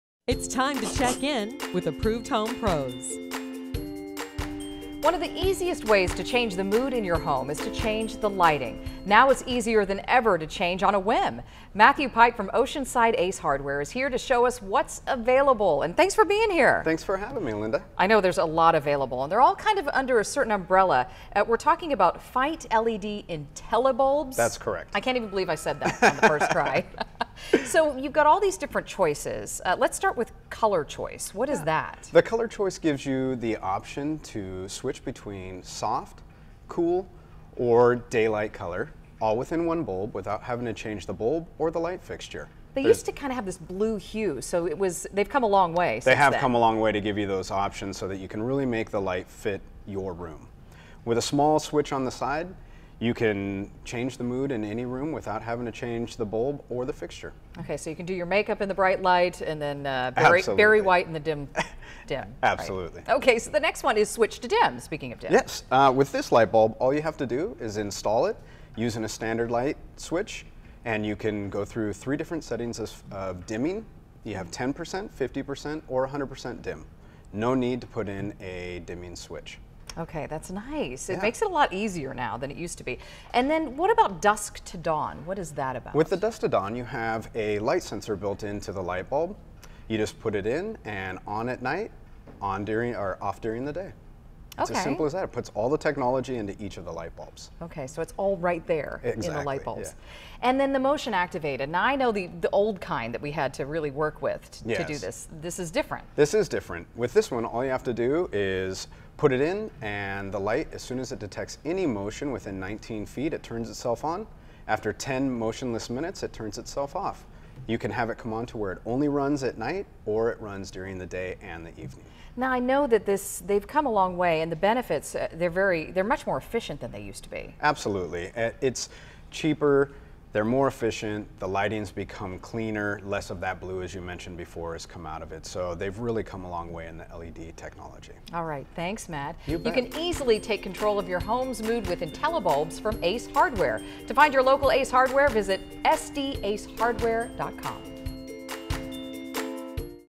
Easily Take Control Of Your Home's Lighting With IntelliBulbs
One of the easiest ways to change the mood in your home is to change the lighting. Oceanside Ace Hardware checks in to share how you can change the lighting in your home on a whim with Feit LED lighting. You can find an extensive selection of IntelliBulbs at your local Ace Hardware.